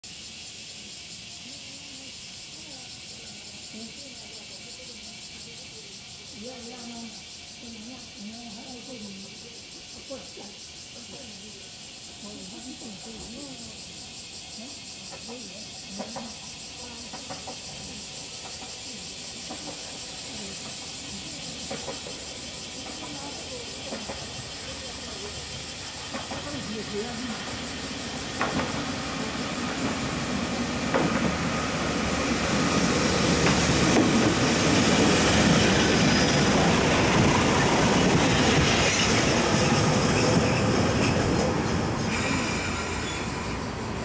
北条鉄道 ～法華口駅行き違い設備完成記念、北条ふらわHM掲出～
法華口駅行き違い設備完成記念『北条ふらわ』の
ヘッドマークが付いた列車を撮りました。
(2020/8/14)